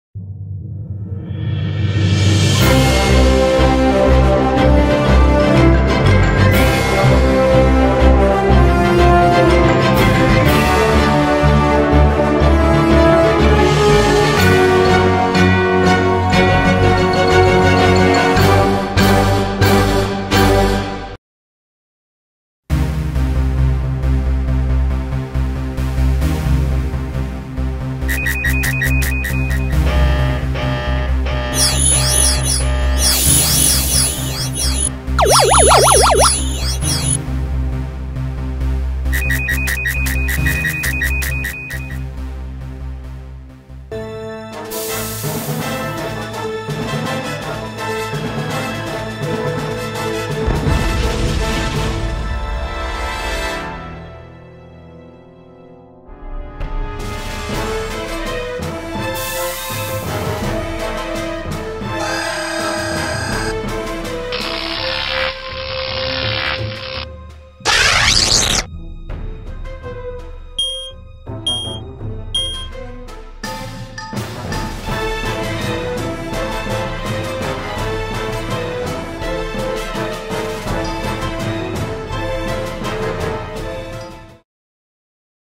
Prokaryotic Transformation Animation
The process of Prokaryotic Transformation told through an exciting science fiction animation.